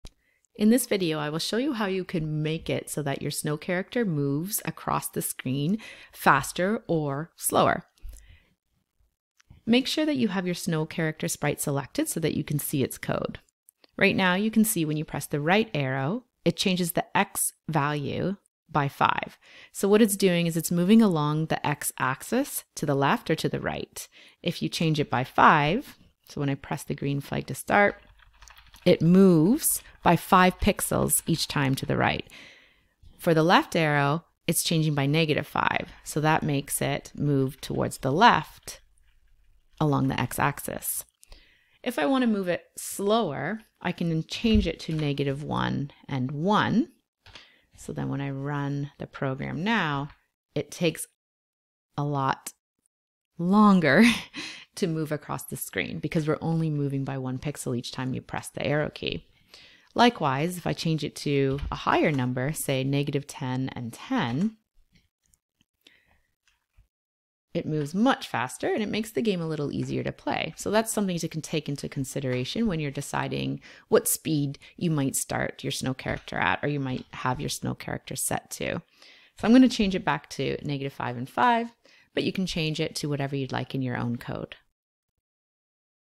Adjusting the speed of a character in a snowball catching Scratch game
I will show you how you can adjust the speed the character moves in a premade Snowball Catching Scratch game.